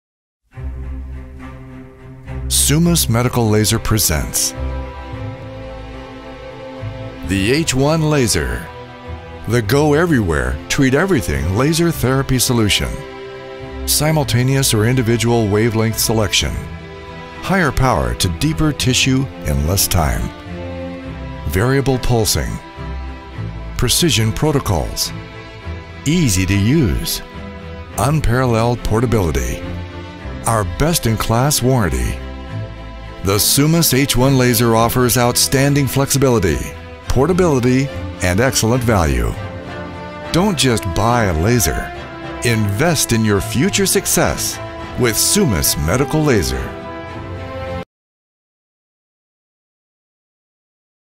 High Power Class 4 Laser Summus Medical Laser H1 in India
The H1 is a lightweight yet sturdy therapy laser that delivers powerful results and simple operation.

15 Watts: Higher power allows you to deliver a therapeutic dose to deeper tissue in less time

3 Wavelengths: Simultaneous or independent application effectively penetrates tissue for excellent results (650nm, 810nm & 915nm)

Pulsing: 1 to 20,000 Hz pulsing range & super pulsing (ISP)
Portable: It only weighs 4.4 pounds
Inbuilt battery: H1 laser comes with inbuilt battery of up to 8 hours (7 hours stationary)

1 Hand Piece Lens: Includes our 25 mm zoom lens

Homogeneous Beam Profile: Delivers the most consistent and safest application
H1 is designed with the highest quality materials and state-of-the-art technology to deliver an excellent therapeutic laser and user experience.

Mobile: (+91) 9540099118 | (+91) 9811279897